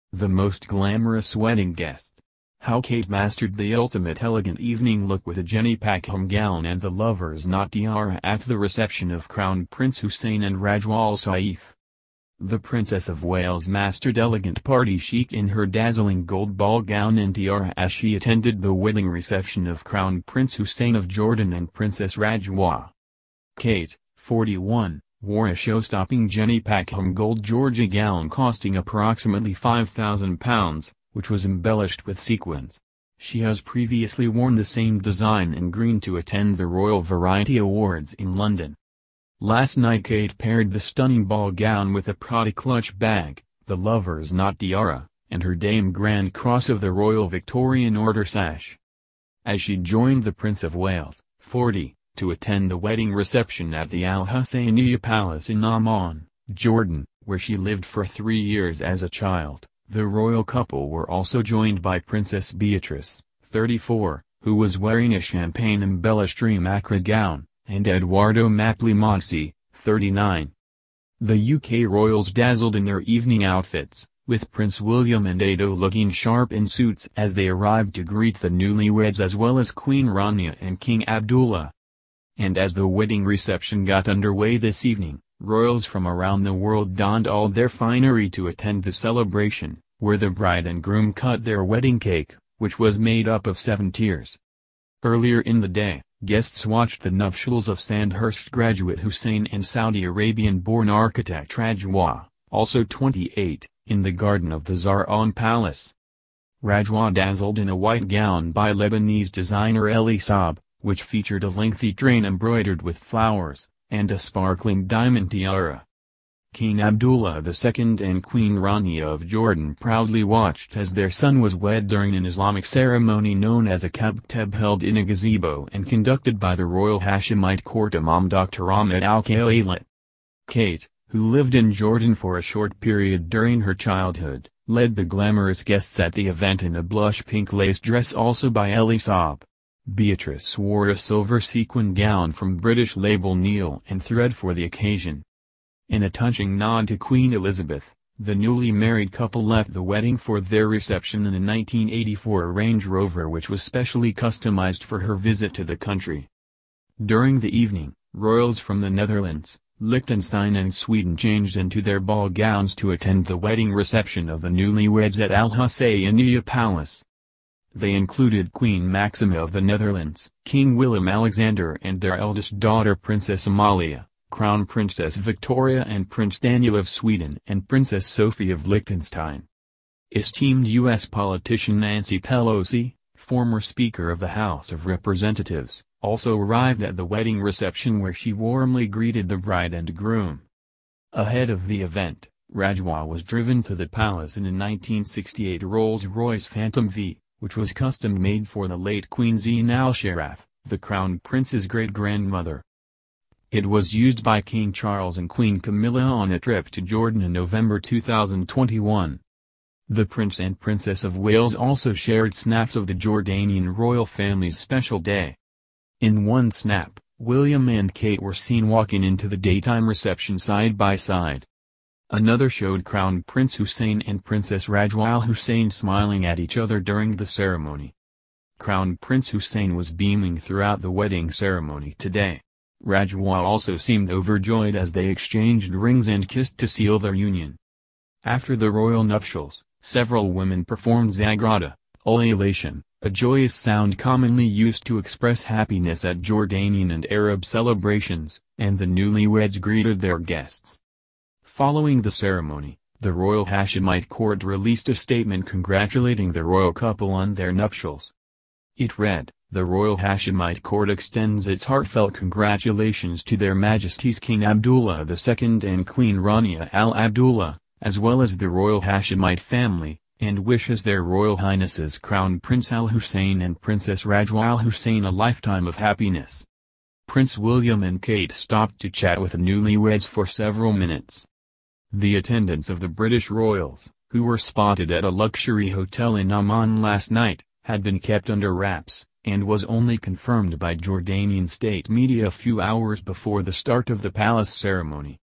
Most glamorous guest! Kate Shines in Cherished Tiara with a Touching Story at Jordan Royal Wedding
The most glamorous guest! Kate Shines in Cherished Tiara with a Touching Story at Jordan Royal Wedding